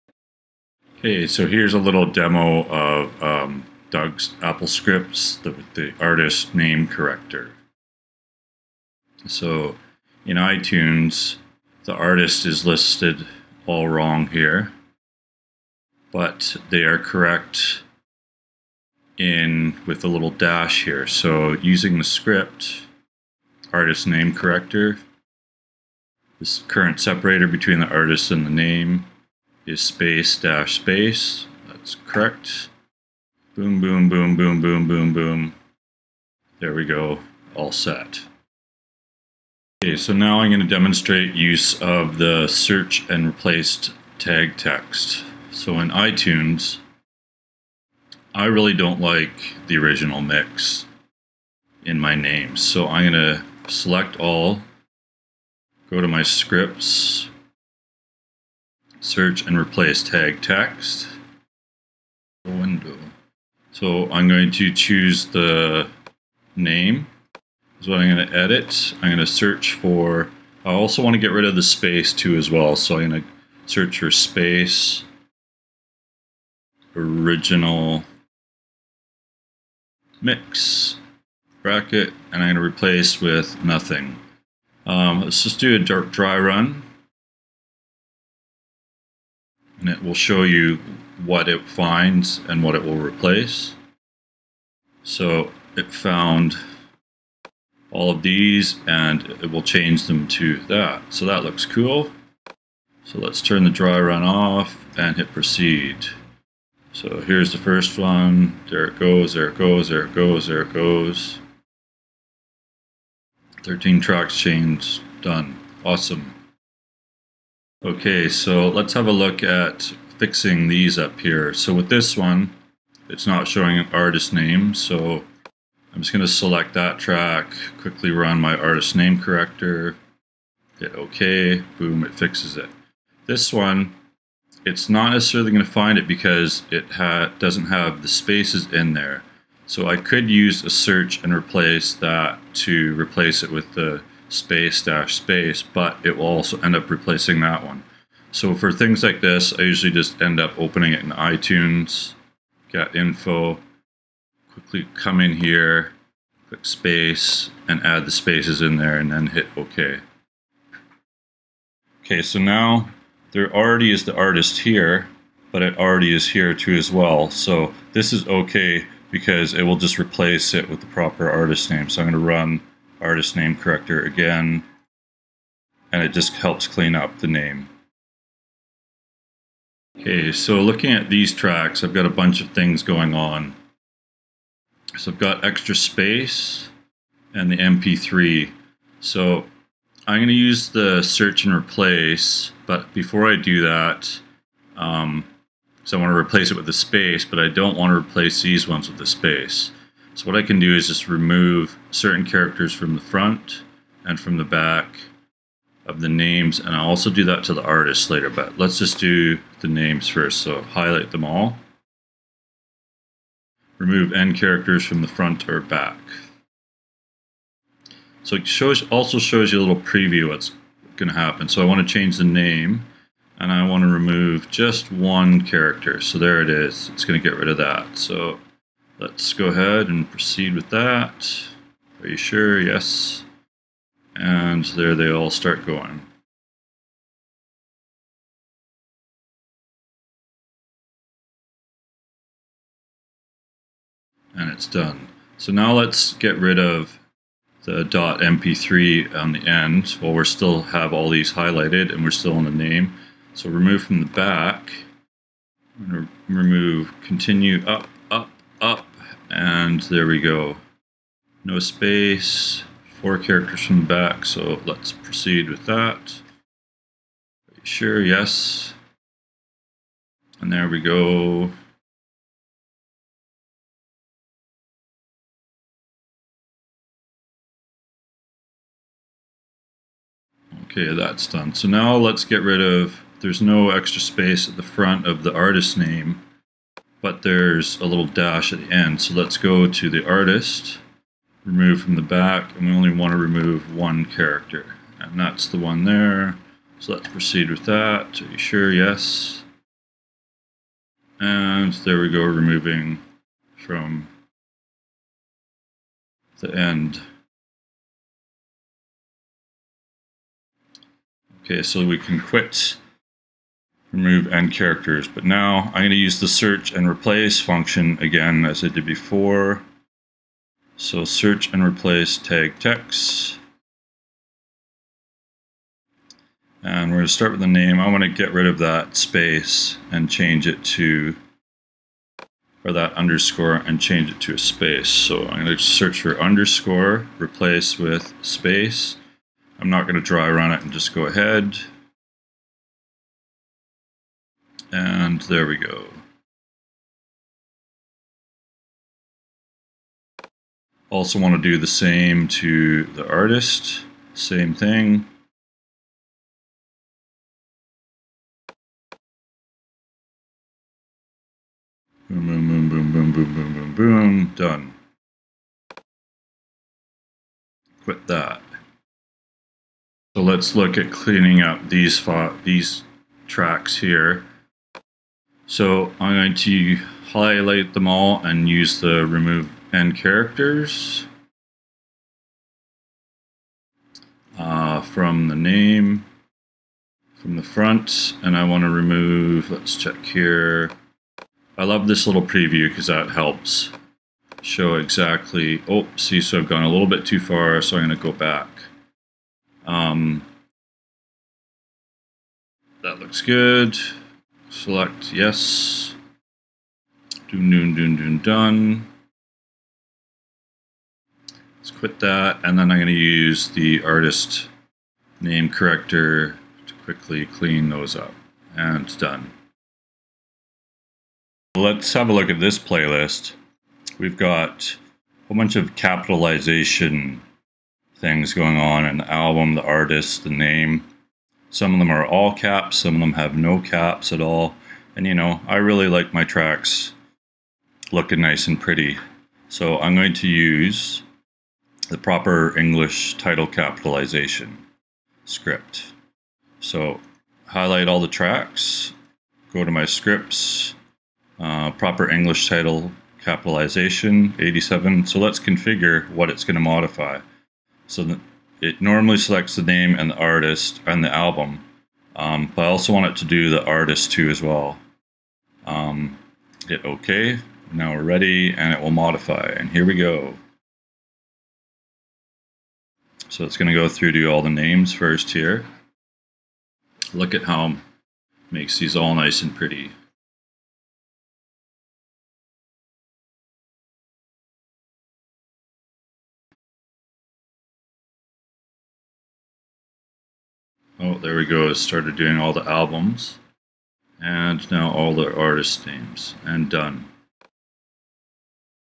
DJ K-Tel's favourite Doug's Scripts for ITunes
DJ K-Tel reviews his favourite tools for fixing Tags in iTunes from Doug's Scripts.
Including Tasks:
- Artist-Name Corector
- Search and Replace Tag Text
- Remove N Characters from the Front or Back
- Proper English Capitalization